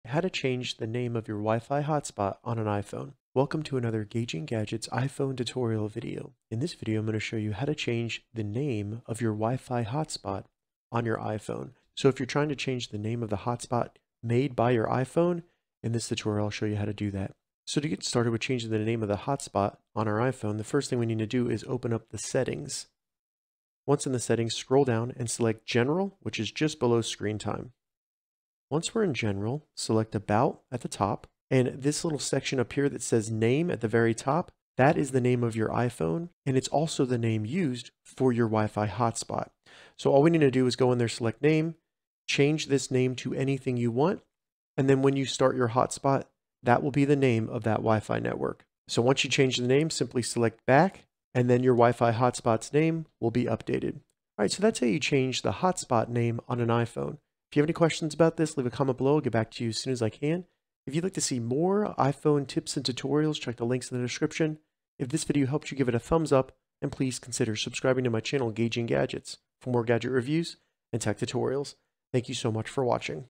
How to Change Hotspot Name on iPhone
How to change Wifi Hotspot name on iPhone? In this tutorial, I show you how to change the name of the iPhone mobile WIFI hotspot. This means if you use the hotspot on your iPhone you can change the Wifi network name of that Hotspot.

Follow the steps below to change hotspot name on iPhone:
1. Open the settings on your iPhone and iPad.
2. In the settings select General.
3. Select About.
4. Select Name.
5. Once in Name, change the text to your desired WiFi hotspot name.
6. Select the back button to save your new hotspot name.